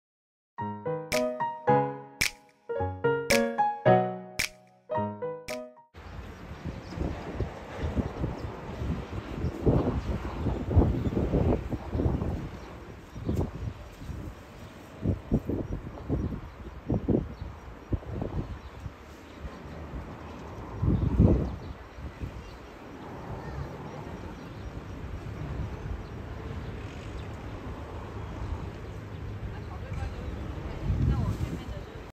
2023/4/ 19 4月份2234次區間車、125次自強號影片
4/19 2234次區間車-基隆
石榴4點08分通過

4/19 125次自強號-屏東

記錄